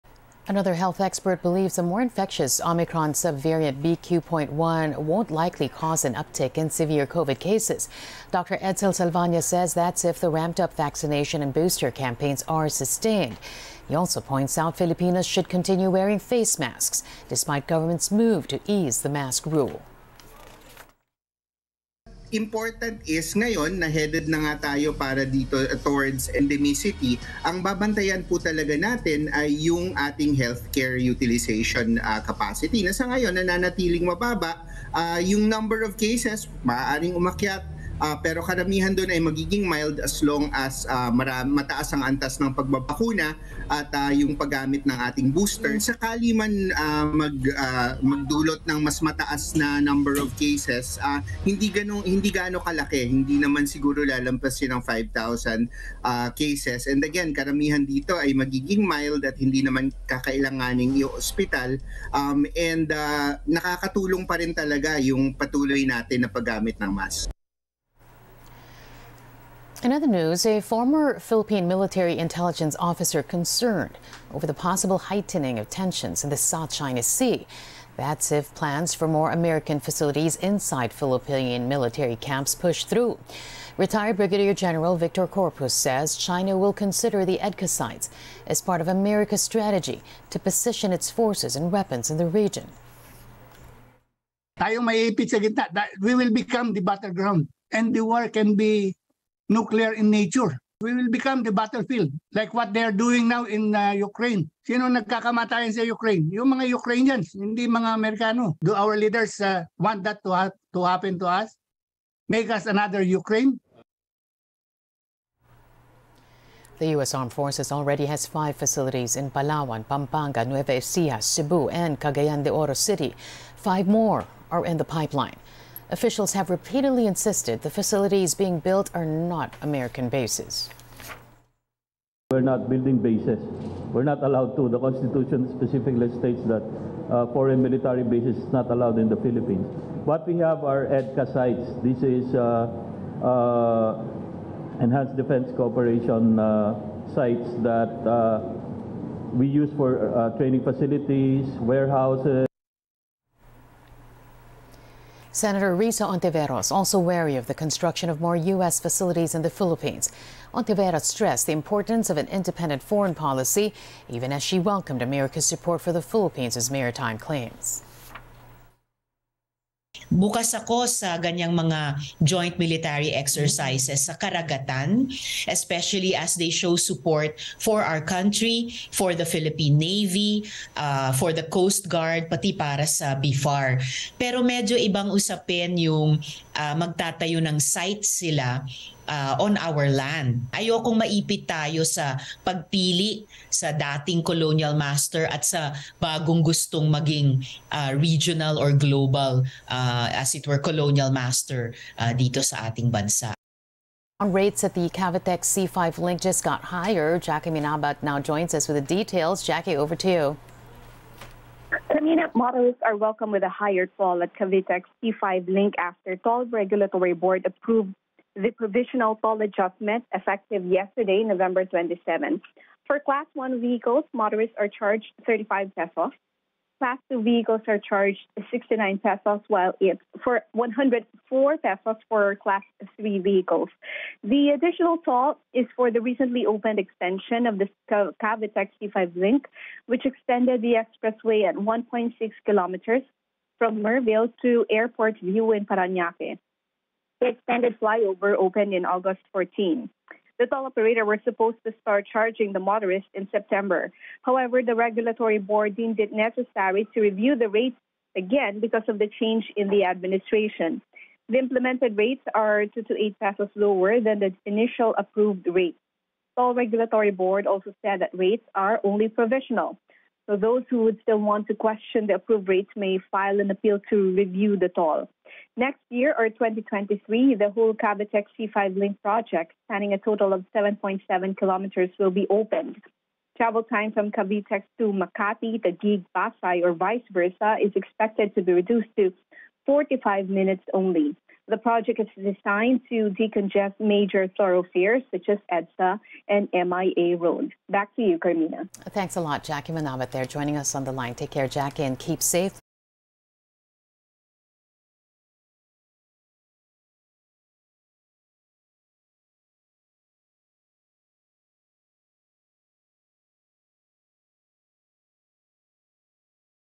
BQ.1 'not seen to cause' uptick in severe COVID cases: expert | ANC
Dateline Philippines: A health expert believes the BQ.1 subvariant won't cause an uptick in severe COVID cases.